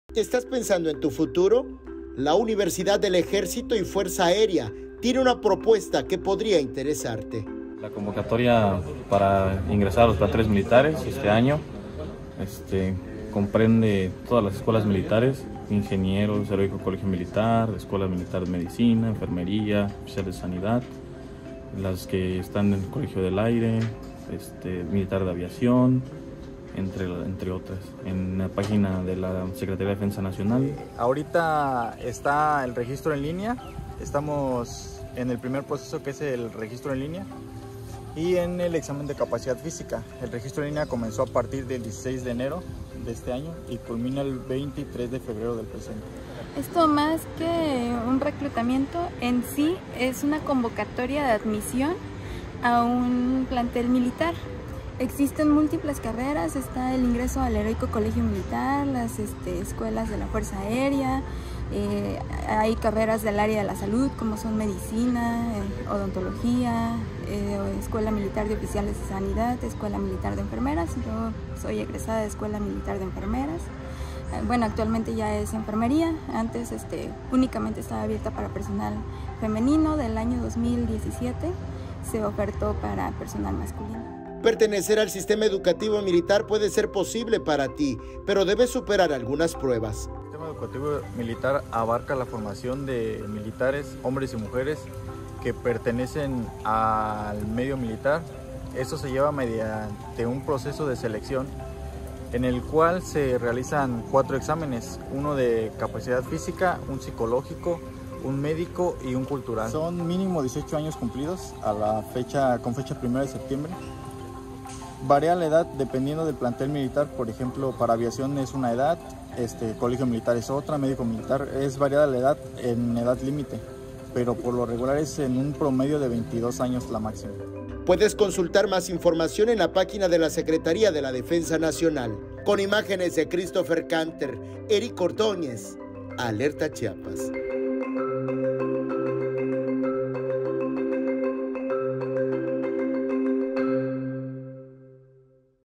¿Quieres pertenecer al Sistema Educativo Militar?
La convocatoria para ingresar a la Universidad del Ejército y Fuerza Aérea estará abierta hasta el próximo 23 de febrero.